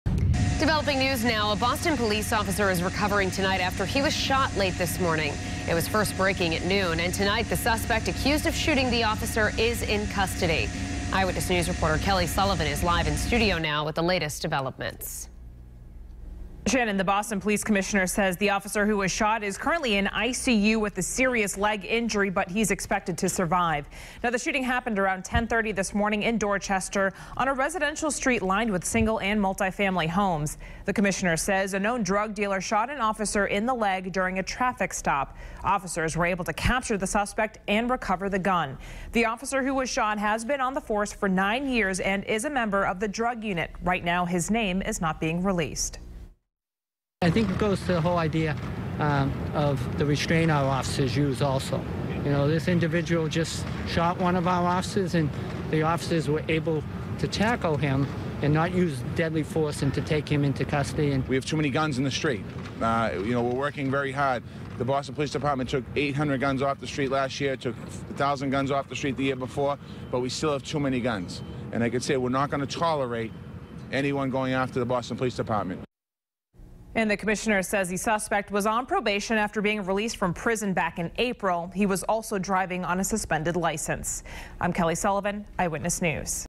Boston police officer shot in leg, is expected to survive
The shooting occurred in the city’s Dorchester neighborhood, on a residential side street lined with single- and multi-family homes.